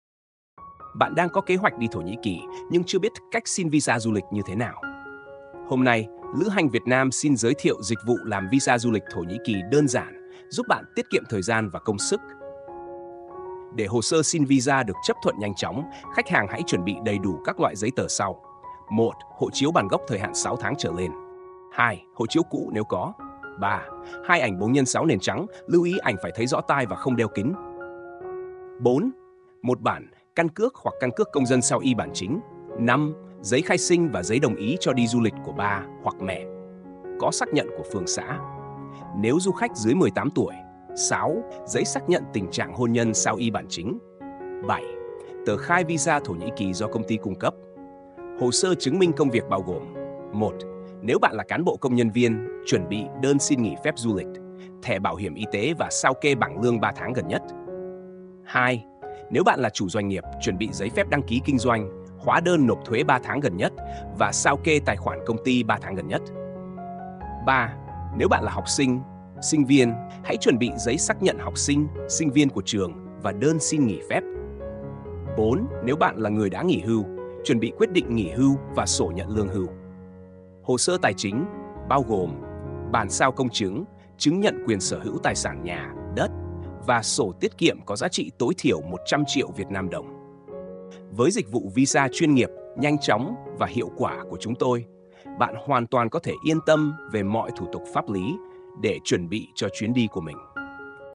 Visa Thổ Nhĩ Kỳ | Hướng dẫn làm Visa Du Lịch Thổ Nhĩ Kỳ
Hướng dẫn thủ tục làm Visa Du Lịch Thổ Nhĩ Kỳ đơn giản, nhanh chóng cùng Lữ hành Việt Nam